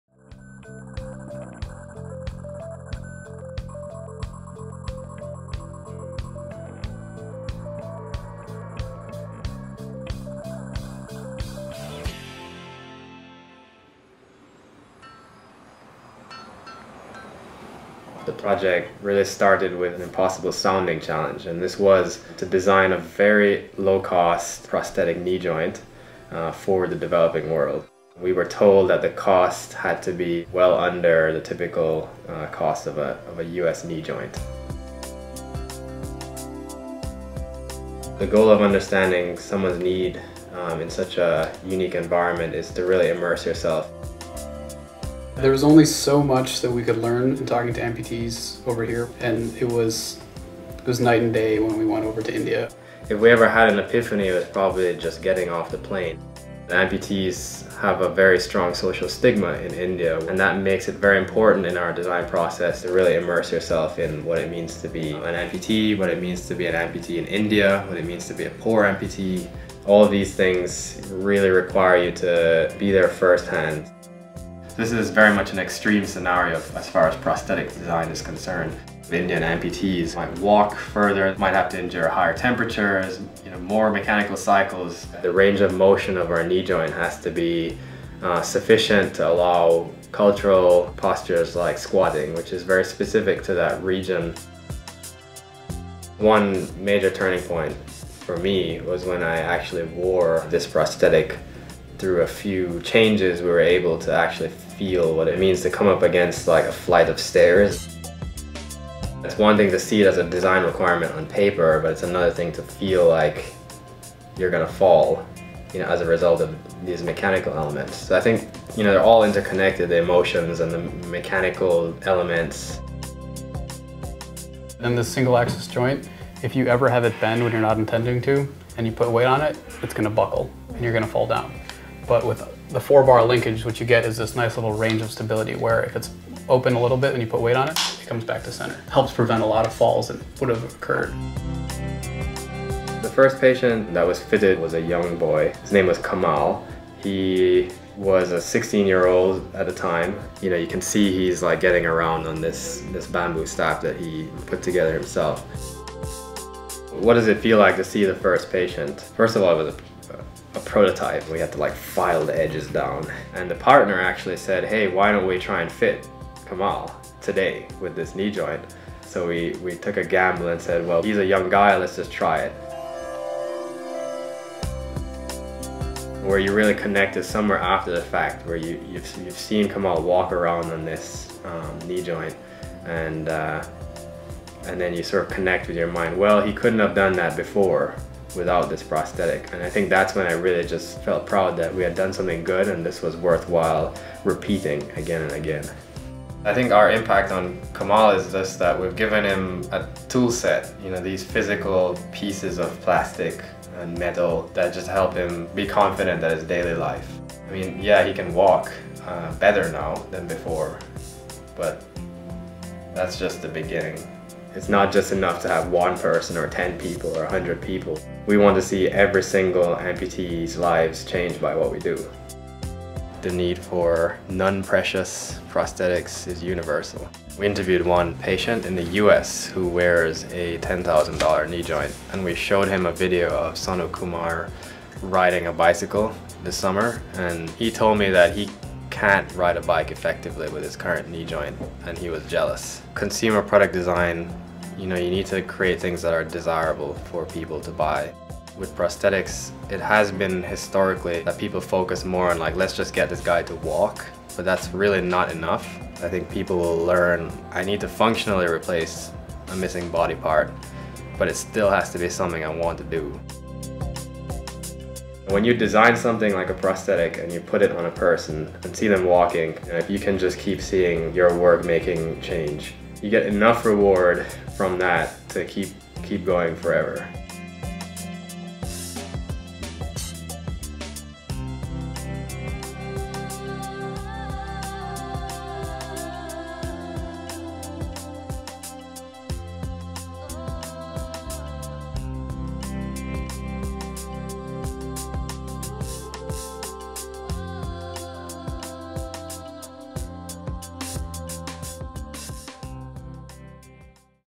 Basset Inspiration Series ReMotion Knee
A group of Stanford students faced an impossible challenge: design an ultra-low cost, high-performance knee joint that is built for extreme conditions of the developing world. Two years later, D-Rev's Japiur (ReMotion) Knee has been fit on over 300 amputees across the globe and highlighted in Time Magazine and Fast Company.